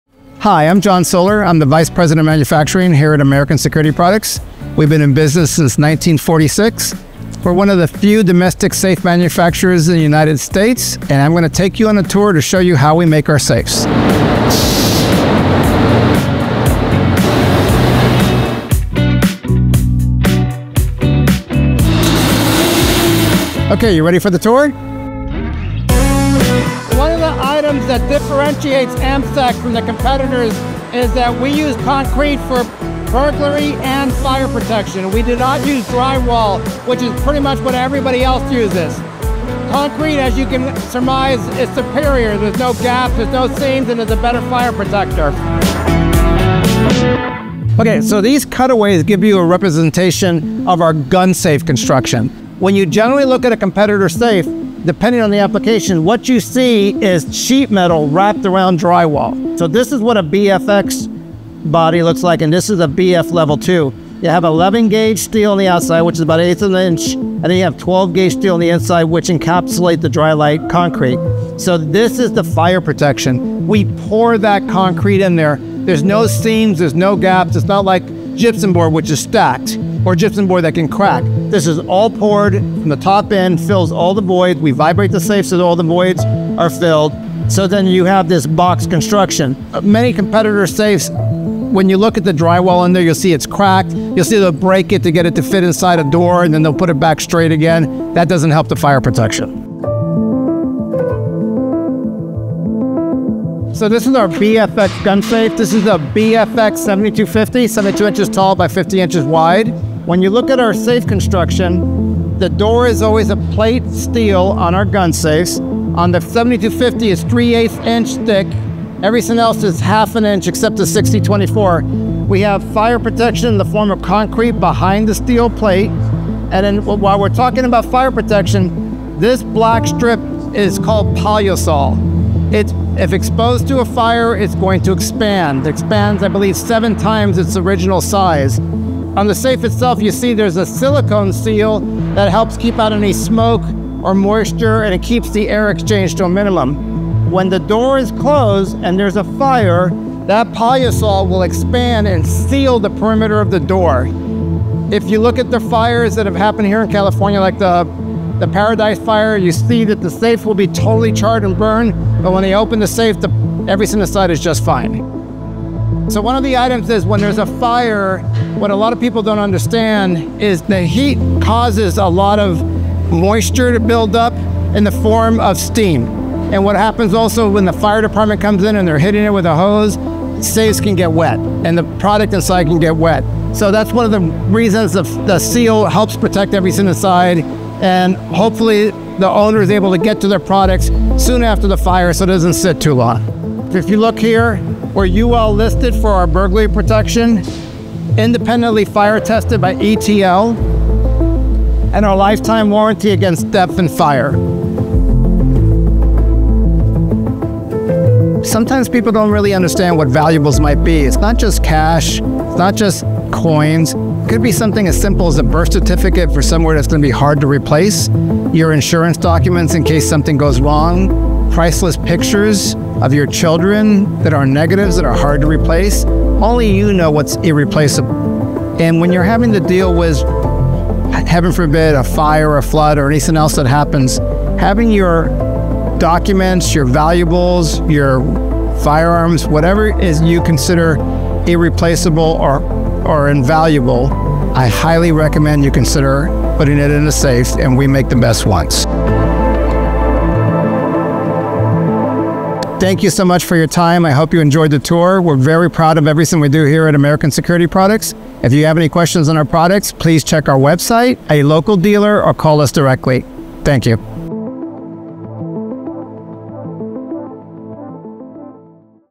What Makes American Security Safes Fire-Resistant? | Exclusive Factory Tour
Discover what makes American Security safes stand out from the competition in this exclusive factory tour! Join John Soler, VP of Manufacturing, as he takes you behind the scenes to show how our fire-resistant, burglary-rated safes are built with concrete instead of drywall for superior protection.

What You’ll Learn in This Video:
✅ The unique construction of American Security's BFX and BFII gun safes
✅ Why concrete is superior to drywall for fire and burglary protection
✅ How Palusol® fire seals expand to protect valuables in a fire 🔥
✅ The importance of UL burglary ratings and ETL fire testing 🔒
✅ How AMSEC safes withstand disasters like California wildfires

Protect your valuables today!